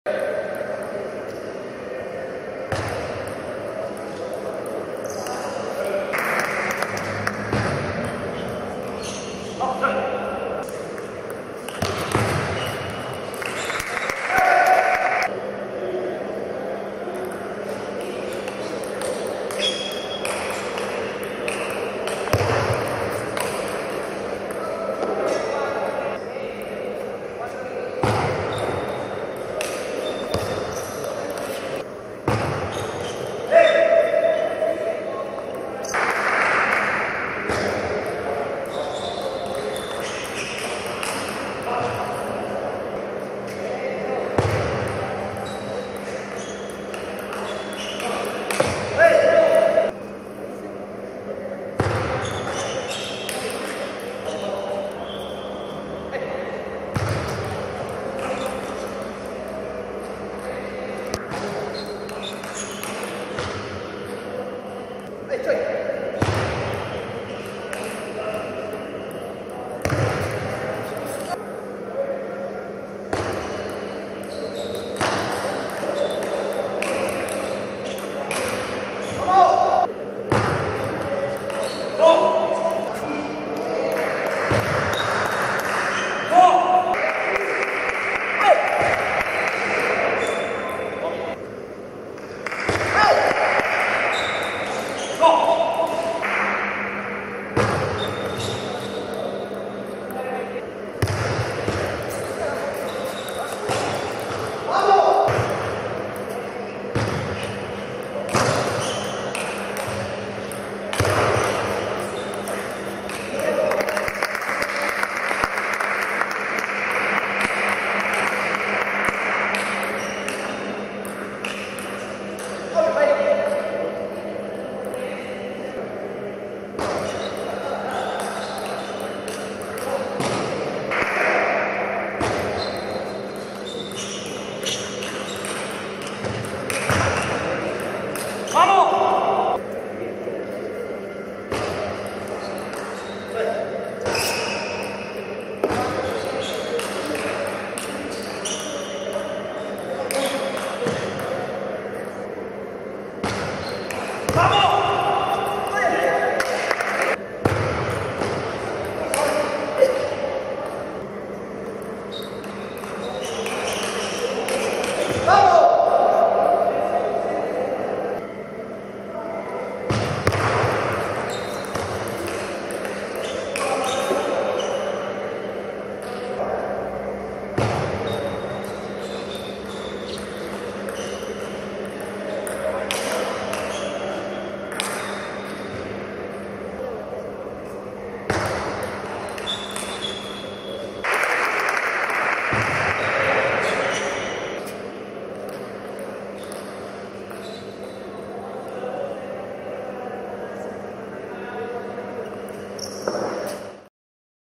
2016 Chile Open Highlights: Sebastian Roman vs Marcelo Fernandez (Pre)
Review all the highlights from the Sebastian Roman vs Marcelo Fernandez (Pre) Match from the 2016 ITTF Chile Open